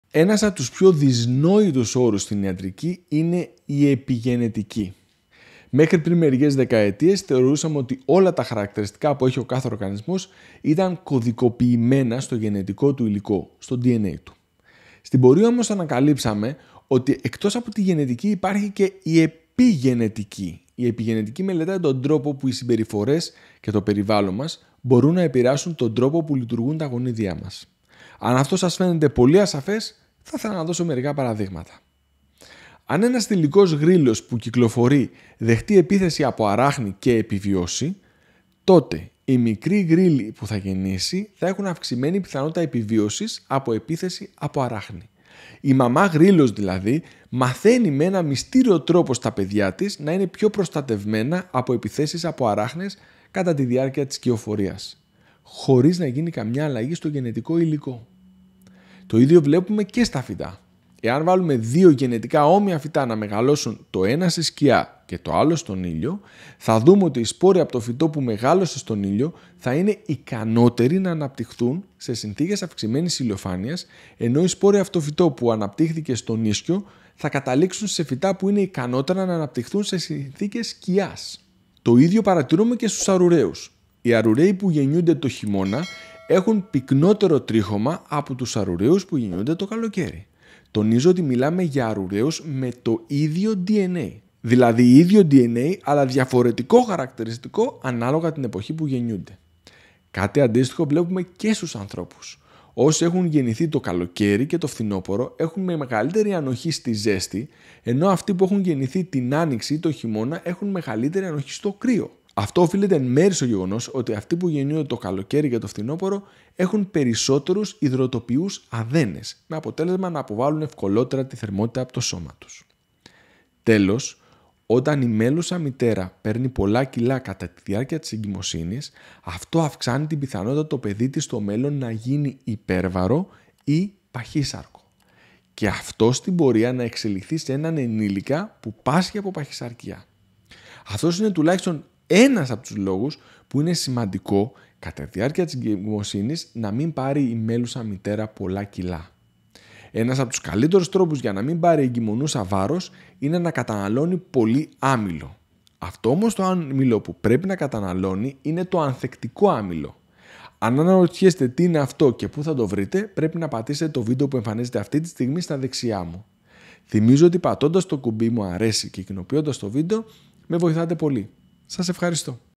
Ποιος αντέχει τη ζέστη;
Ένας απ’ τους πιο δυσνόητους όρους στην ιατρική είναι η επιγενετική. Μέχρι πριν μερικές δεκαετίες θεωρούσαμε ότι όλα τα χαρακτηριστικά που έχει ο κάθε οργανισμός ήταν κωδικοποιημένα στο γενετικό του υλικό, στο DNA του. Στην πορεία όμως ανακαλύψε με ότι εκτός από τη γενετική υπάρχει και η επιγενετική. Η επιγενετική μελετάει τον τρόπο που οι συμπεριφορές και το περιβάλλον μας μπορούν να επηρεάσουν τον τρόπο που λειτουργούν τα γονίδια μας. Αν αυτό σας φαίνεται πολύ ασαφές, θα ήθελα να δώσω μερικά παραδείγματα.

Αν ένας θηλυκός γρύλος που κυκλοφορεί δεχτεί επίθεση από αράχνη και επιβιώσει, τότε οι μικρή γρύλοι που θα γεννήσει θα έχουν αυξημένη πιθανότητα επιβίωσης από επίθεση από αράχνη. Η μαμά γρύλος δηλαδή μαθαίνει με έναν μυστήριο τρόπο στα παιδιά της να είναι πιο προστατευμένα από επιθέσεις από αράχνες κατά την διάρκεια της κυοφορίας. Χωρίς να γίνει καμία αλλαγή στο γενετικό υλικό.

Το ίδιο βλέπουμε και στα φυτά. Εάν βάλουμε 2 γενετικά όμοια φυτά να μεγαλώσουν το ένα στη σκιά και το άλλο στον ήλιο θα δούμε ότι οι σπόροι από το φυτό που μεγάλωσε στον ήλιο θα είναι ικανότεροι να αναπτυχθούν σε συνθήκες αυξημένης ηλιοφάνειας,  ενώ οι σπόροι από το φυτό που αναπτύχθηκε στον ίσκιο θα καταλήξουν σε φυτά που είναι ικανότερα να αναπτυχθούν σε συνθήκες σκιάς.

Το ίδιο παρατηρούμε και στους αρουραίους. Οι αρουραίοι που γεννιούνται τον χειμώνα έχουν πυκνότερο τρίχωμα από τους αρουραίους που γεννιούνται το καλοκαίρι. Τονίζω ότι μιλάμε για αρουραίους με το ίδιο DNA! Άρα, ίδιο DNA, αλλά διαφορετικό χαρακτηριστικό ανάλογα την εποχή που γεννιούνται.

Κάτι αντίστοιχο βλέπουμε και στους ανθρώπους. Όσοι έχουν γεννηθεί το καλοκαίρι και το φθινόπωρο έχουν μεγαλύτερη ανοχή στη ζέστη, ενώ αυτοί που έχουν γεννηθεί την άνοιξη ή τον χειμώνα έχουν μεγαλύτερη ανοχή στο κρύο. Αυτό οφείλεται εν μέρει στο γεγονός ότι αυτοί που γεννιούνται το καλοκαιρι και το φθινόπωρο έχουν περισσότερους ιδρωτοποιούς αδένες, με αποτέλεσμα να αποβάλλουν ευκολότερα την θερμότητα από το σώμα τους.

Τέλος, όταν η μέλλουσα μητέρα παίρνει πολλά κιλά κατά τη διάρκεια της εγκυμοσύνης αυτό αυξάνει την πιθανότητα το παιδί της στο μέλλον να γίνει υπέρβαρο ή παχύσαρκο και αυτό στην πορεία να εξελιχθεί σε έναν ενήλικα που πάσχει από παχυσαρκία. Αυτός είναι τουλάχιστον ένας από τους λόγους που είναι σημαντικό κατά τη διάρκεια της εγκυμοσύνης να μην πάρει η μέλλουσα μητέρα κιλά.

Ένας από τους καλύτερους τρόπους για να μην πάρει η εγκυμονούσα βάρος είναι να καταναλώνει πολύ άμυλο. Αυτό όμως το άμυλο που πρέπει να καταναλώνει είναι το ανθεκτικό άμυλο. Αν αναρωτιέστε τι είναι αυτό και που θα το βρείτε, πρέπει να πατήσετε το βίντεο που εμφανίζεται αυτή τη στιγμή στο δεξιά μου.

Θυμίζω ότι πατώντας το κουμπί μου αρέσει και κοινοποιώντας το βίντεο με βοηθάτε πολύ. Σας ευχαριστώ.